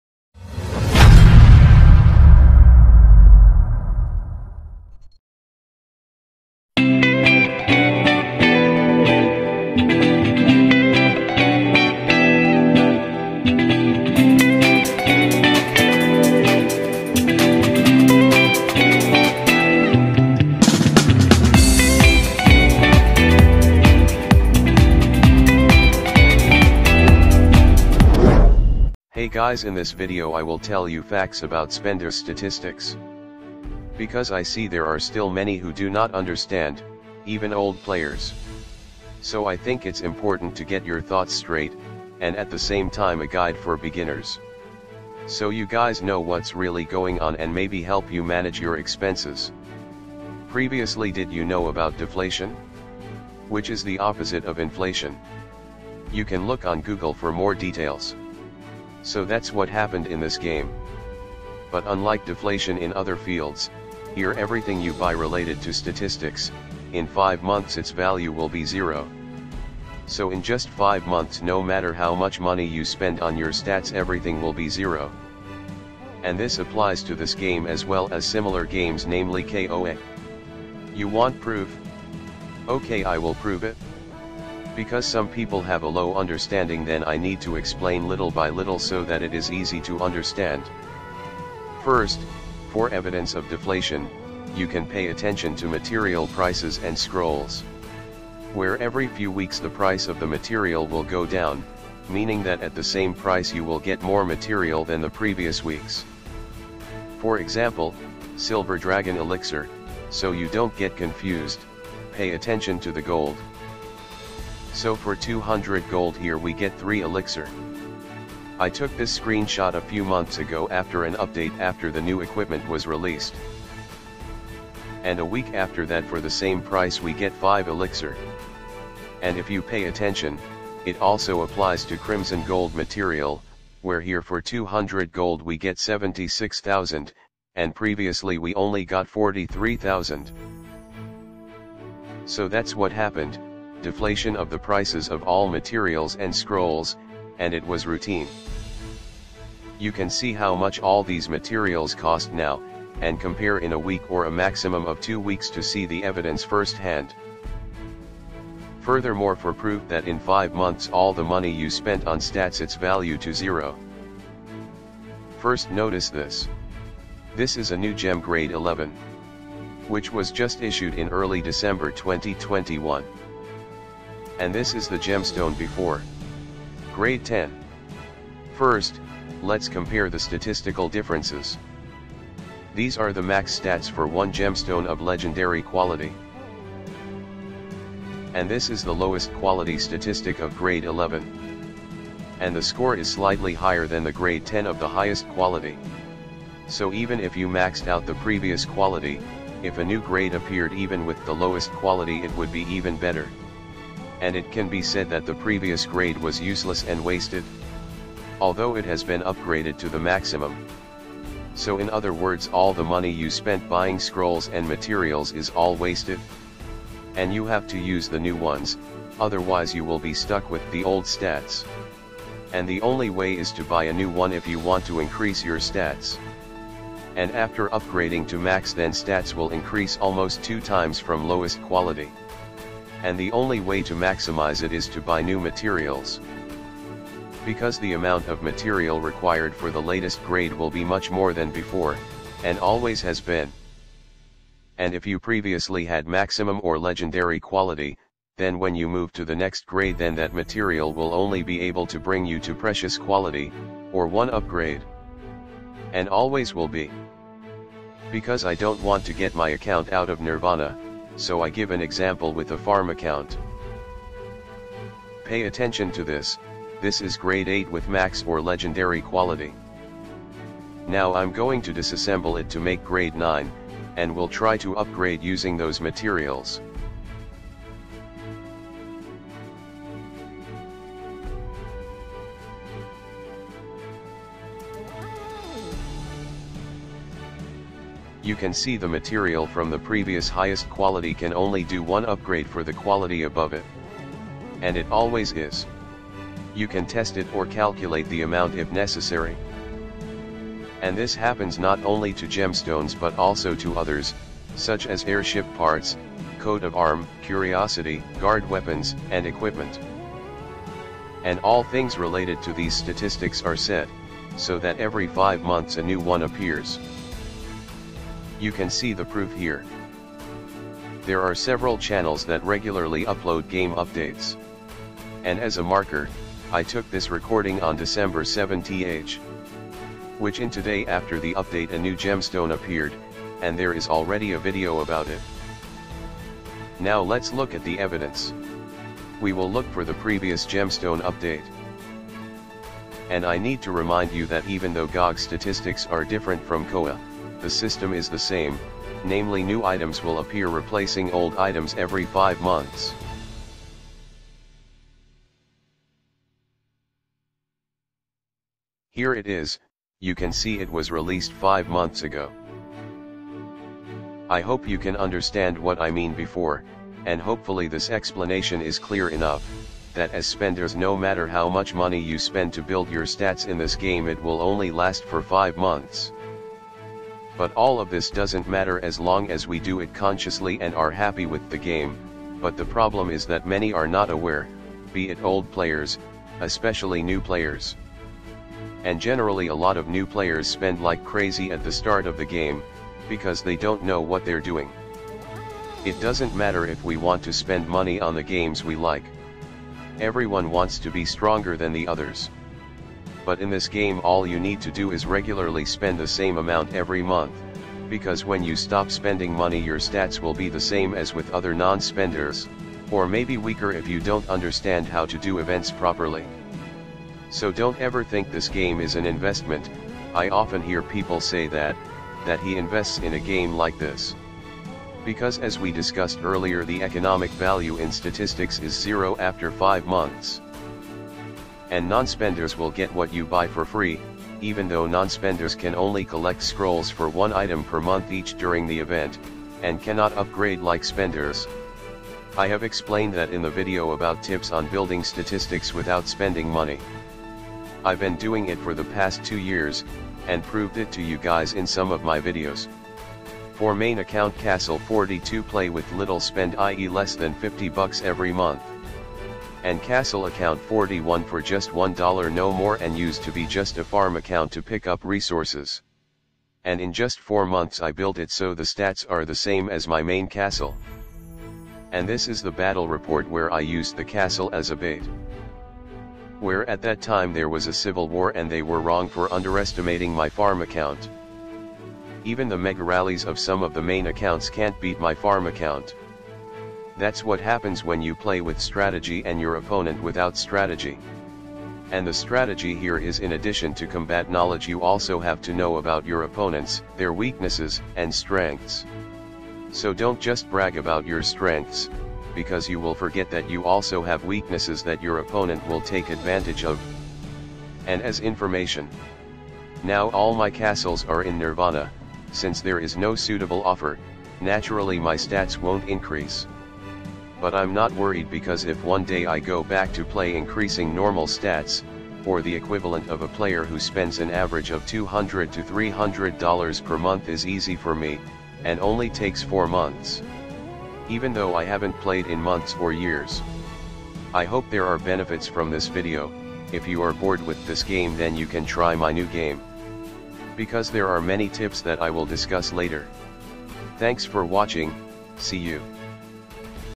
spendors stats facts on Guns of glory and king of Avalon. are you sure you already know this fact?
this is only information based on facts and data. and that's what's happened for the past few years, maybe there will be a change someday, maybe.🤷